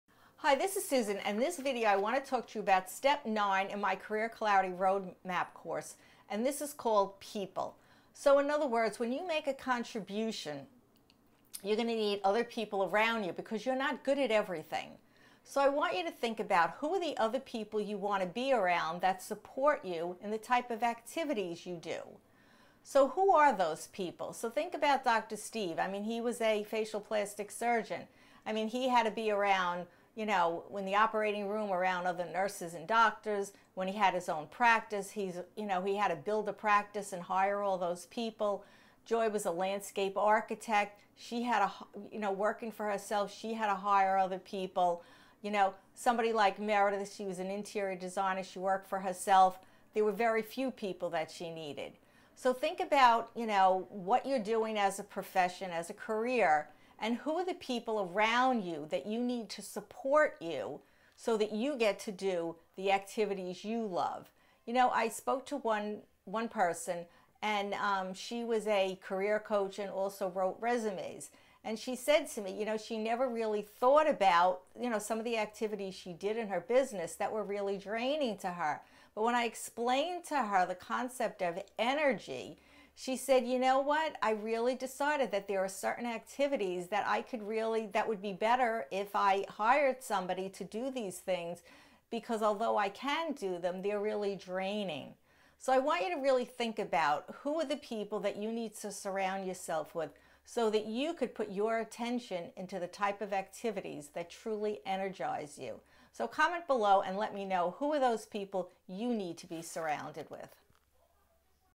How to Find Your PASSION Step 9 People Career Clarity Roadmap
Once you are clear on the contribution you are making and your daily activities you have to complete the picture with people around you. That is your co-works, peers and customers.

Do you need other people to help you make your contribution? You can't do everything you are going to need other people to do the things you don’t enjoy. Who are the people you need to create your contribution?
What about people and relationships. Do you like having relationships with people at work? Do you need that energy?
Even in terms of thinking about structuring there's a lot of structuring that can be done even when you work by yourself. When you work alone you are not alone you still have to interact with a lot of other people. You can help other people. You have to choose which clients you want. There is a lot of decisions to make when you look at your schedule there's a lot of structuring involved. You are choosing the people you interact with and the relationships you form with other.

II. People Support Your Contribution
In Module 2B I talked about the circumstances that bring you together with your customer. What else can you tell me about them? You are going to take a deeper look into your customer.
Think about the person who likes being behind the scenes and supporting the leader. Who is the person you want to support?

The people you would interact with on a daily basis based on what you want to do in your career. It is your relationship with the people you are interacting with at work.

Paul chose his career because he wanted to be around people who wanted to have a huge impact on the world. Paul chose to work for famous authors marketing their workshops and speaking events all over the world. He knew the type of person that motivated him to support.

Secrets to Finding a Career You Love
Your Passion
Your Purpose
Your Profession
Feel the energy of doing what you LOVE